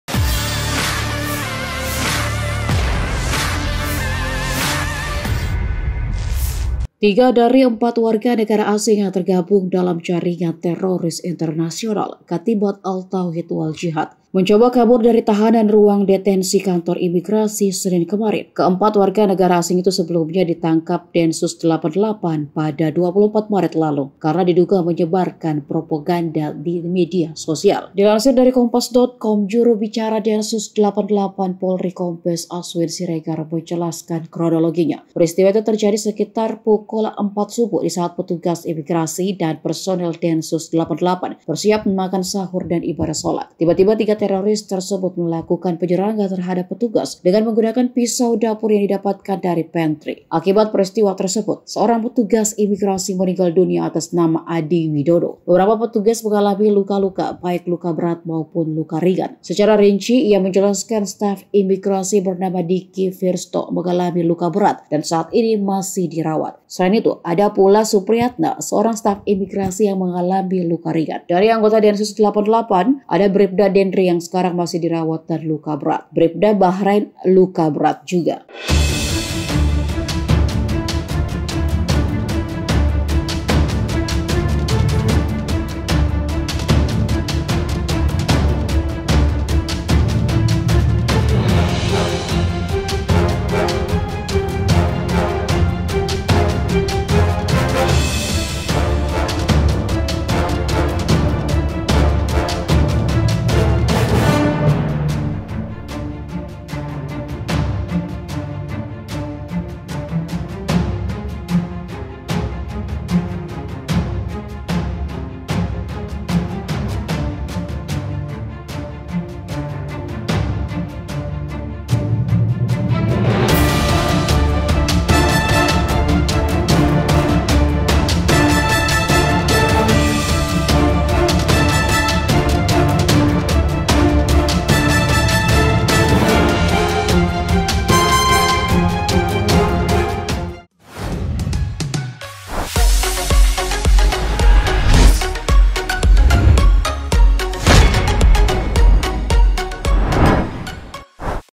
Anggota Densus 88 & Pegawai Imigrasi Diserang 3 WNA Teroris Internasional saat Sahur, Satu Tewas
TRIBUN-TIMUR.COM - 3 dari empat Warga negara asing yang tergabung dalam jaringan teroris internasional Katibat al Tauhid wal Jihad, berhasil kabur dari tahanan ruang detensi Kantor Imigrasi senin kemarin.

Dilansir dari Kompas.com, Juru Bicara Densus 88 AT Polri Kombes Aswin Siregar menjelaskan kronologinya.

Peristiwa itu terjadi sekitar pukul 04.00 WIB, disaat petugas Imigrasi dan personel Densus 88 bersiap makan sahur dan ibadah salat.

Tiba-tiba 3 teroris tersebut melakukan penyerangan terhadap petugas dengan menggunakan pisau dapur yang didapatkan dari pantry.

Akibat peristiwa tersebut, seorang petugas imigrasi meninggal dunia atas nama Adi Widodo

beberapa petugas mengalami luka-luka, baik luka berat maupun luka ringan.

Secara rinci, ia menjelaskan, staf imigrasi bernama Dikky Firstho mengalami luka berat dan saat ini masih dirawat.

Selain itu, ada pula Supriatna, seorang staf imigrasi yang mengalami luka ringan.

dari anggota Densus 88 ada Bripda Dendri yang sekarang masih dirawat dan luka berat, Bripda Bahrain luka berat

Beruntung kini seluruh WNA tersebut telah ditangkap kembali dan ditahan dengan pengamanan super ketat.